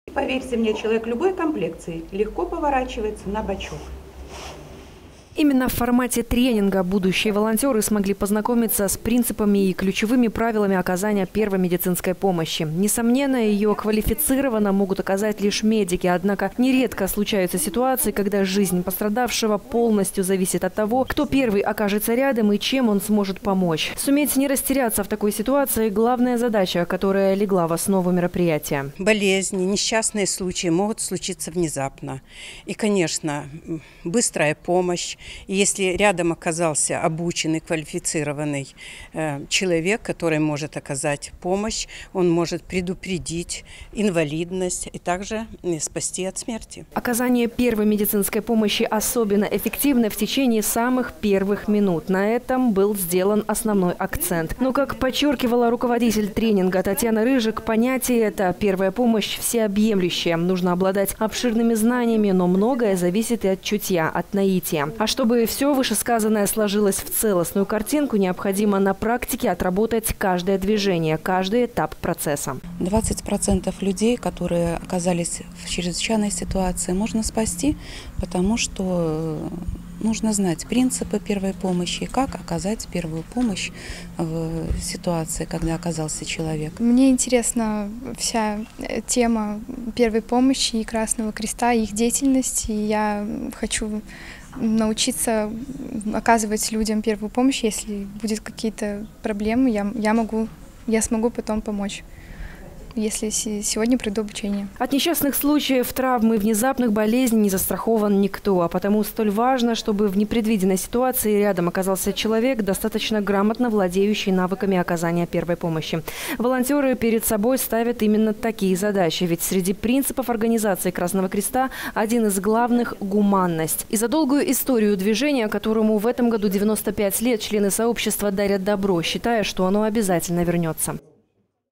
2016-09-09 г. Брест. Тренинг волонтеров Красного Креста. Новости на Буг-ТВ.
Тренинг волонтеров Красного Креста, приуроченный к Международному дню оказания первой медпомощи.
10-ого сентября весь мир отметит день оказания первой медицинской помощи. Не остается в стороне от события и город над Бугом. Сегодня в брестском доме ветеранов состоялся обучающий и познавательный тренинг для будущих волонтеров Красного Креста в плане оказания неотложной помощи. О том, чему научились юные добровольцы, подробнее в сюжете.